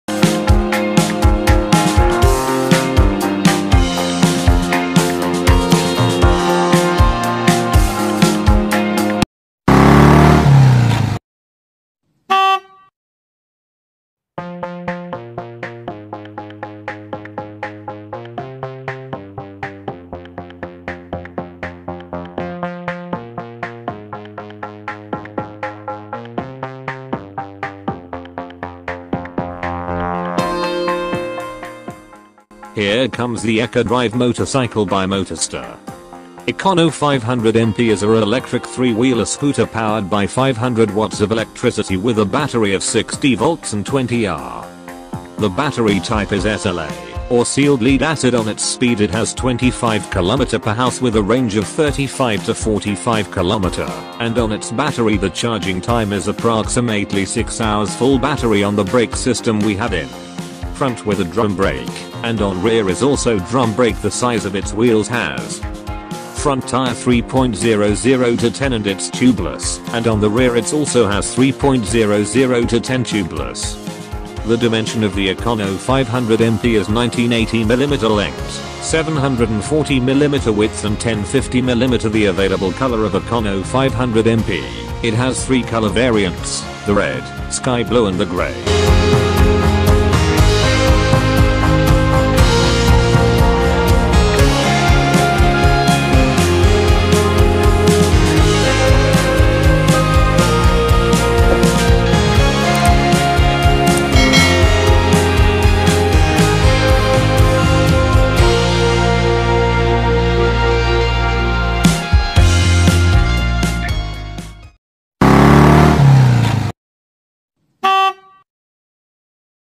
ECODRIVE ECONO 500 MP 2023 PRICE SPECS FEATURE AND COLOR
Here comes the ecodrive motorcycle by motorstar.
Econo 500 mp
is a electric three wheeler scooter Powered by 500 watts of electricity with a
Battery of 60volts and 20ah..